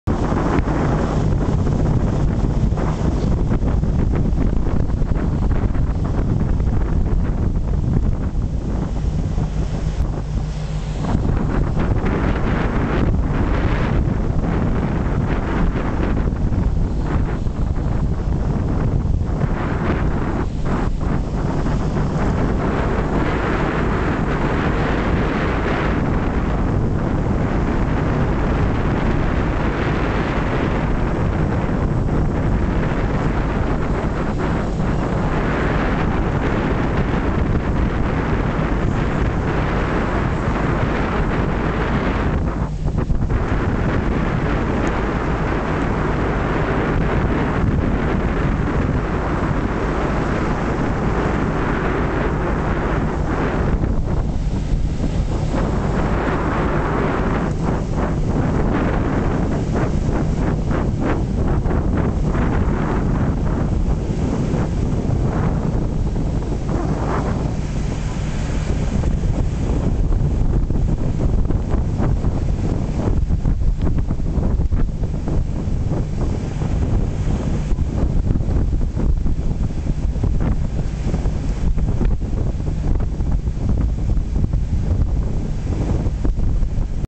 Meltdown !
On travelling by ship through the Neumayer Channel in Antarctica we were left awe-struck by the calving of a glacier right in front of our eyes. The newly born iceberg created a mini tsunami as it settled into its transformed existence. We were able to capture the meltdown on video.

 Submitted by: Anna O'Keeffe